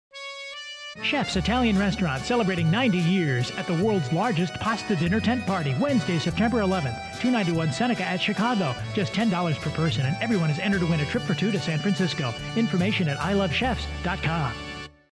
Chef's Restaurant - The World's Largest Pasta Dinner Party
WBBZ-TV Production
Editor: Joe Maulucci

The World's Largest Pasta Dinner Party
Buffalo, New York
Wednesday, September 11th at 6pm